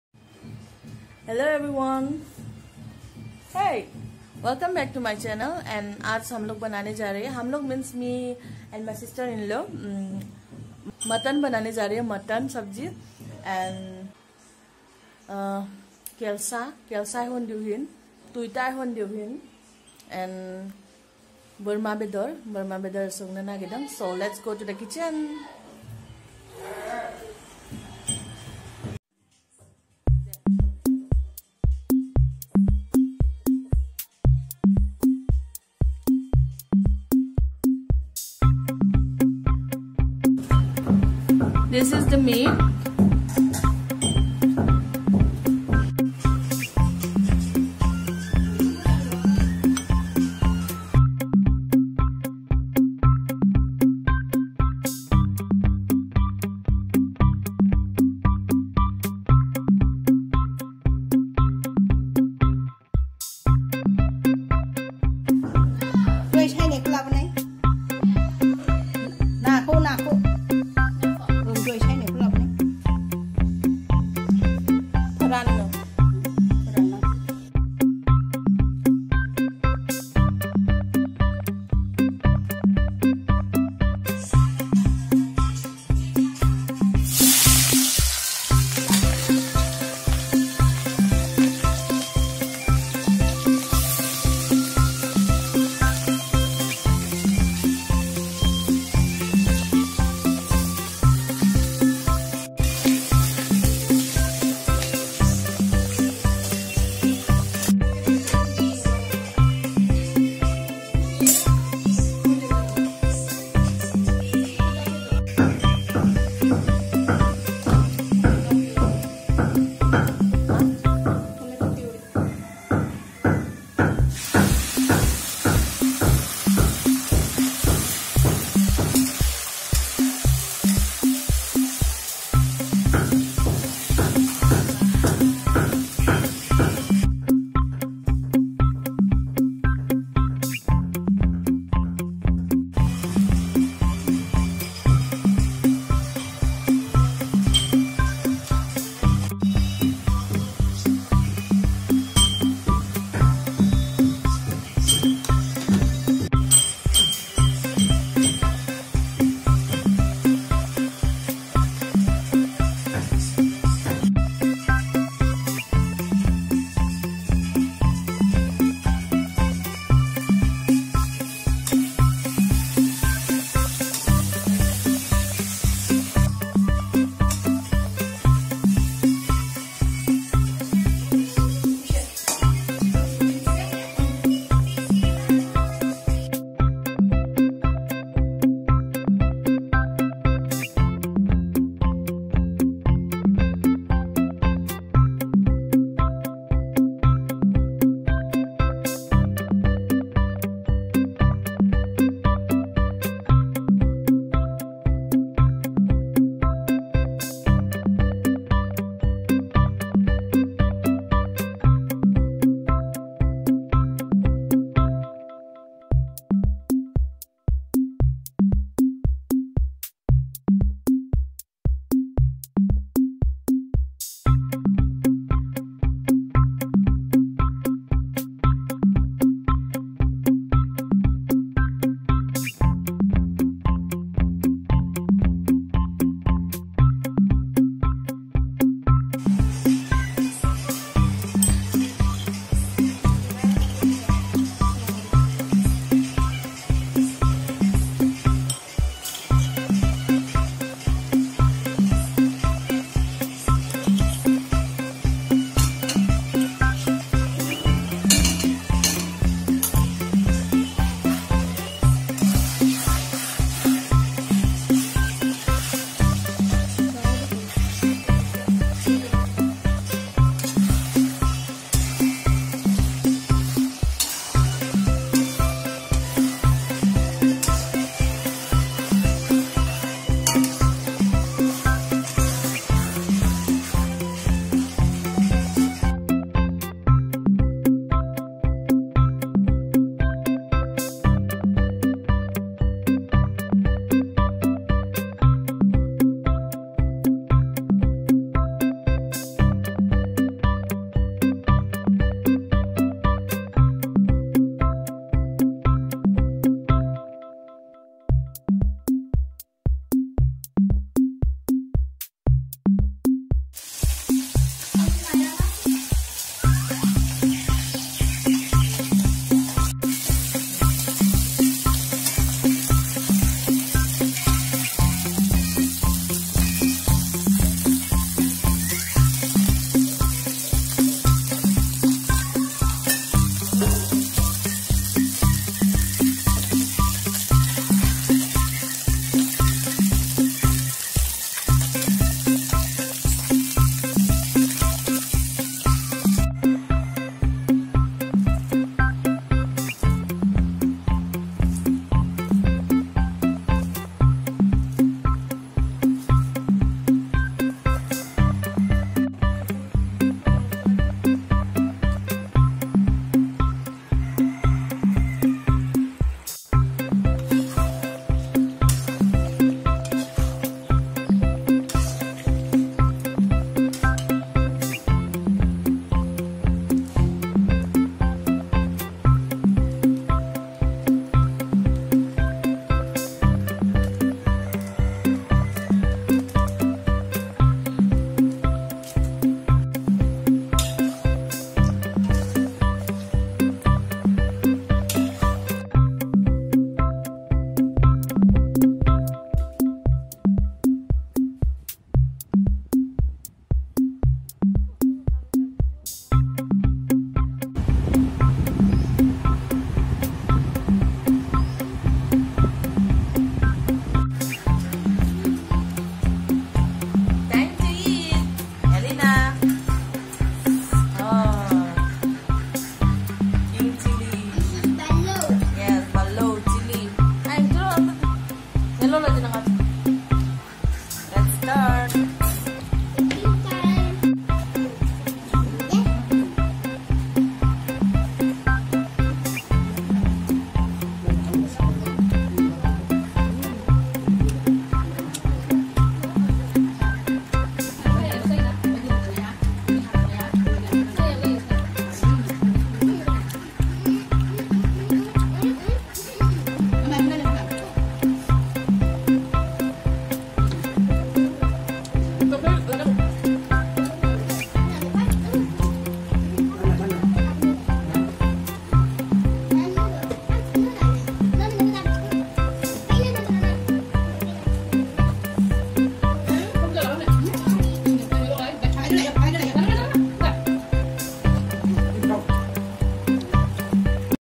TASTY MUTTON CURRY/SOMY HANGSING GOYARI
A very easy and tasty mutton recipe